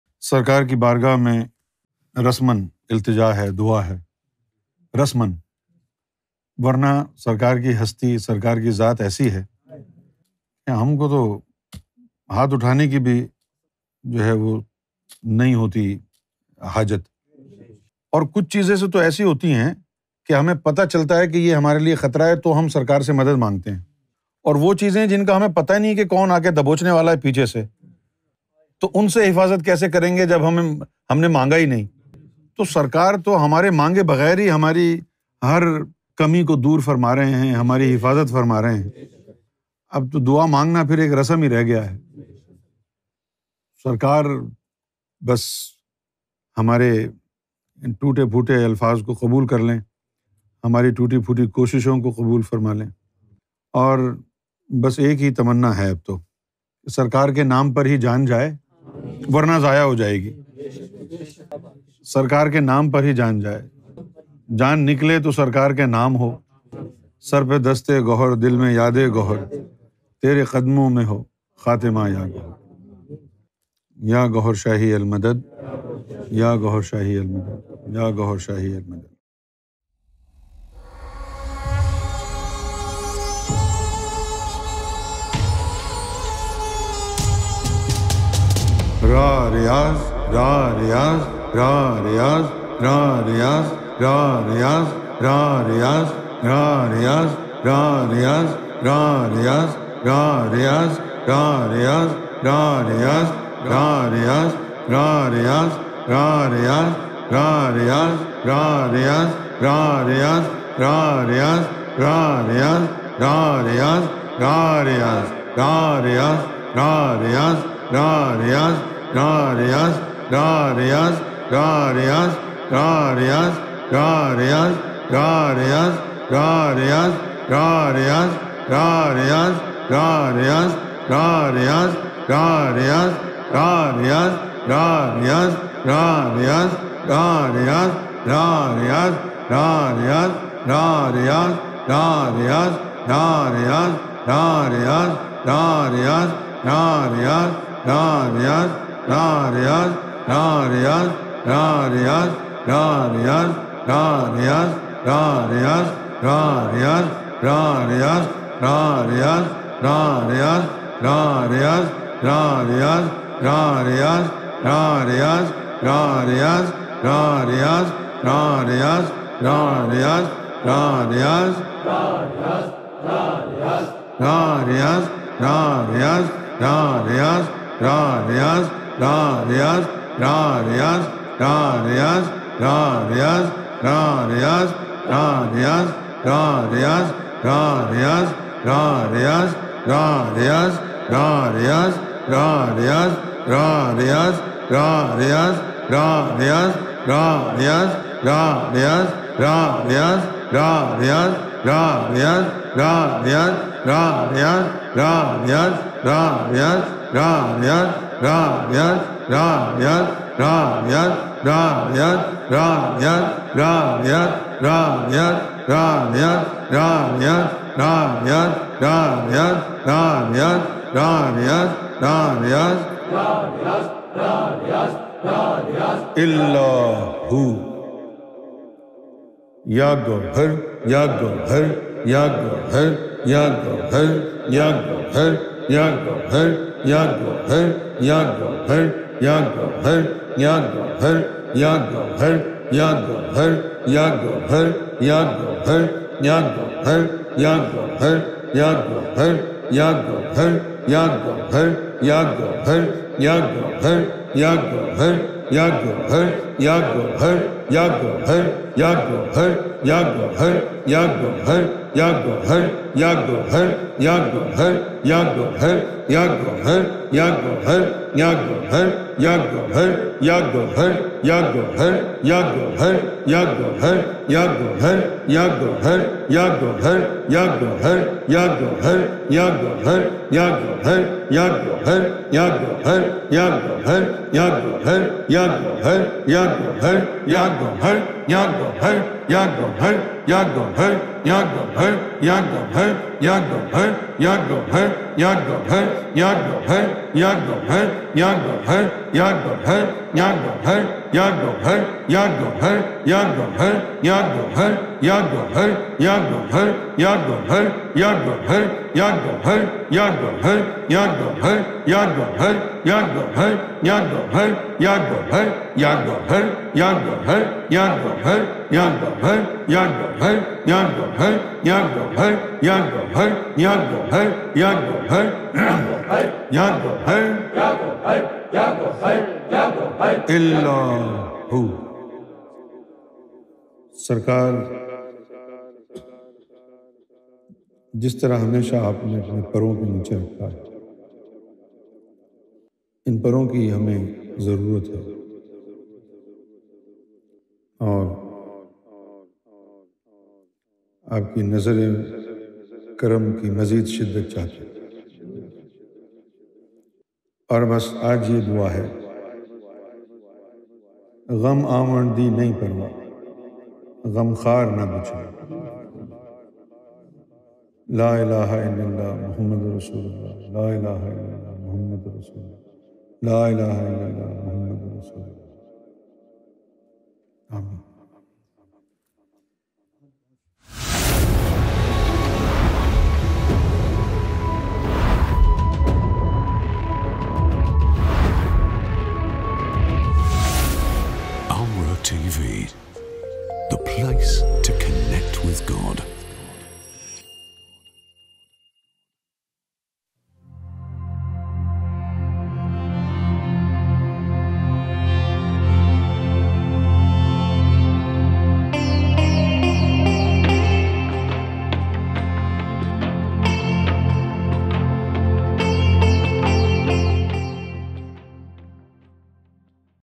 Youm e Ghaibat: Khaas Mehfil e Zikr | Younus AlGohar | ALRA TV
A special Zikr mehfil held on the Day of Occultation (2022).

Sufi Master Younus AlGohar leads a special zikr mehfil and delivers a heartfelt prayer in the Divine Court of His Divine Eminence Gohar Shahi.